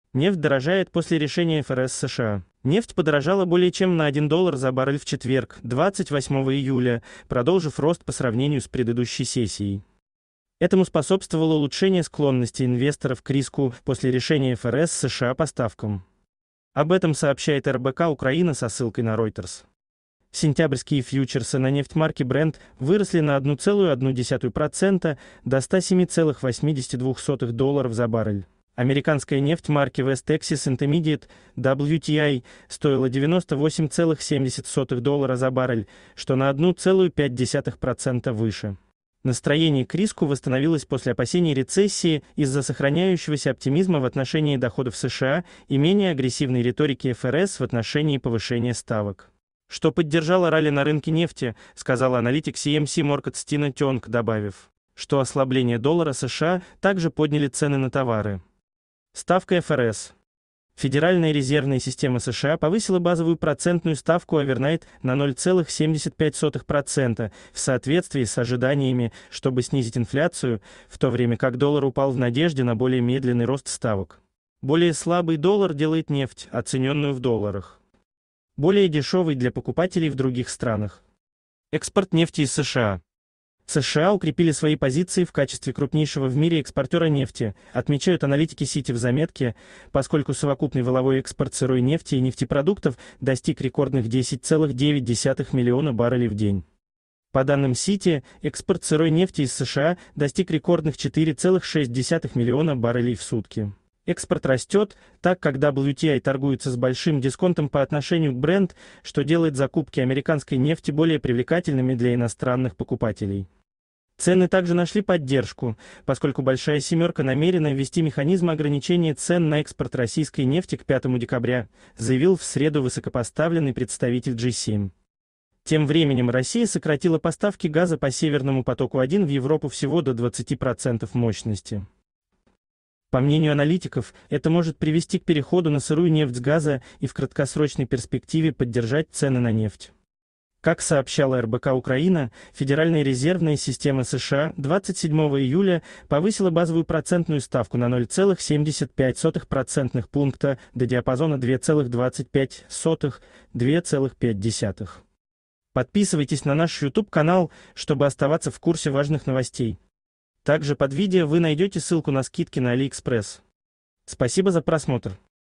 Нефть дорожает после решения ФРС США.
Нефть Получи скидку на Алиэкспресс по этой ссылке до 70% + дополнительные скидочные купоны от продавцов. Нефть подорожала более чем на 1 доллар за баррель в четверг, 28 июля, продолжив рост по сравнению с предыдущей сессией. Этому способствовало улучшение склонности инвесторов к риску после решения ФРС США по ставкам. Об этом сообщает РБК-Украина со ссылкой на Reuters. Сентябрьские фьючерсы на нефть марки Brent выросли на 1,1%, до 107,82 долларов за баррель. Американская нефть марки West Texas Intermediate (WTI) стоила 98,70 доллара за баррель, что на 1,5% выше. "Настроение к риску восстановилось после опасений рецессии из-за сохраняющегося оптимизма в отношении доходов США и менее агрессивной риторики ФРС в отношении повышения ставок, что поддержало ралли на рынке нефти", - сказала аналитик CMC